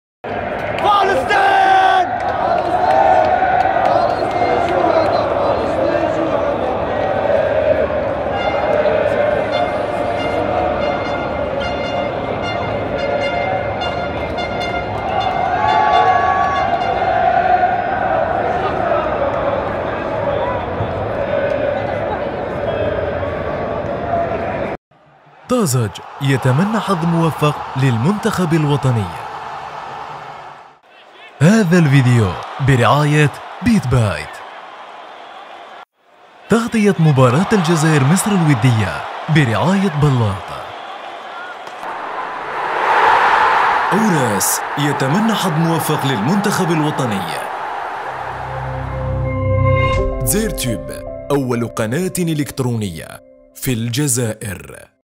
شااهد ... قبل إنطلاق المباراة الودية ... فلسطين الشهداء تدوي بقوة ملعب هزاع بن زايد بالإمارات
شااهد ... قبل إنطلاق المباراة الودية ... فلسطين الشهداء تدوي بقوة ملعب هزاع بن زايد بالإمارات ... أسمع